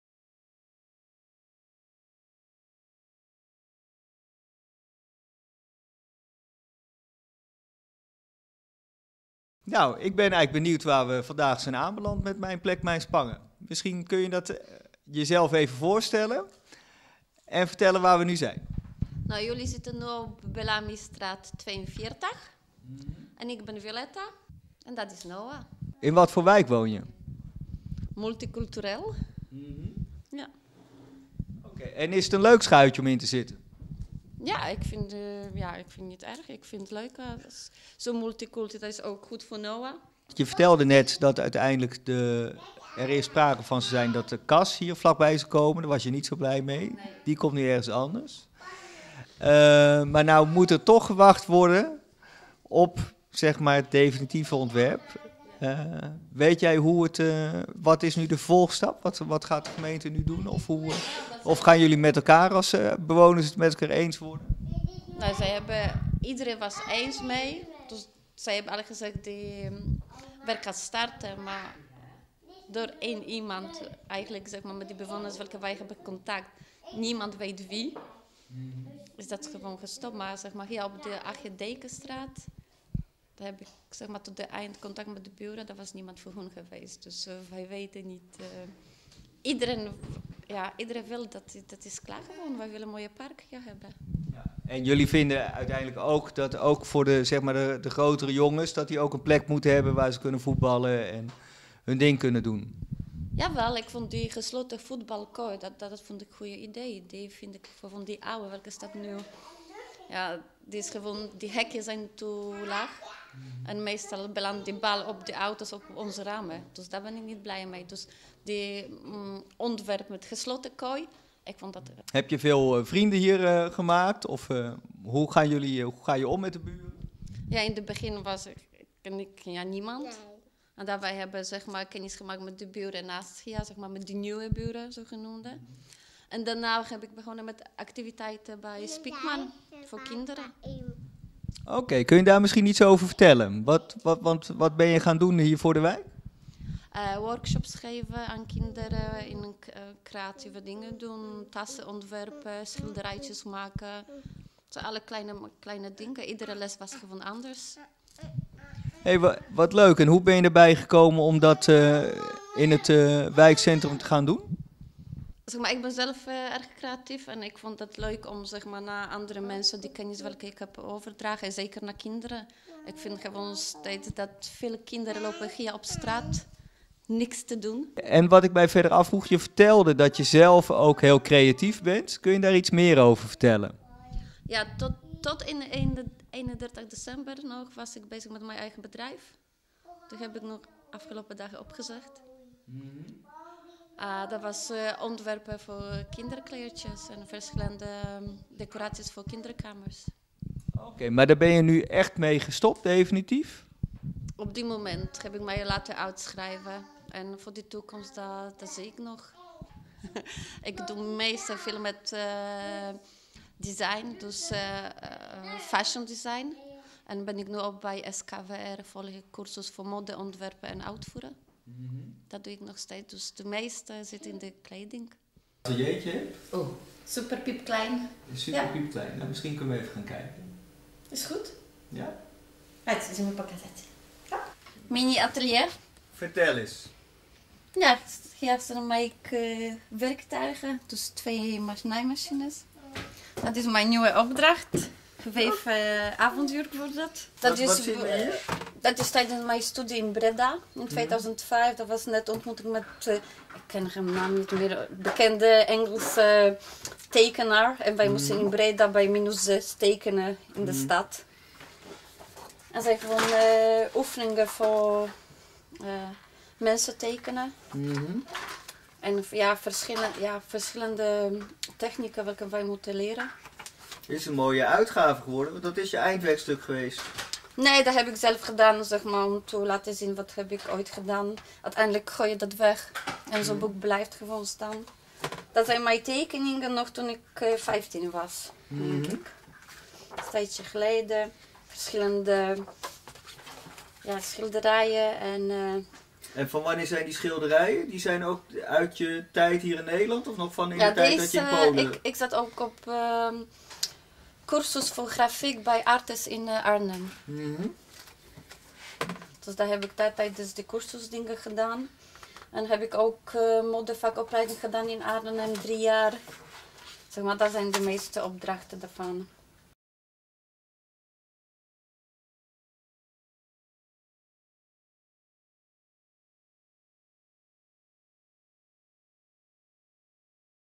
Mijn plek mijn violetta
Violetta Riedell woont met veel plezier in Spangen, en werkt zelf met textiel en soms ook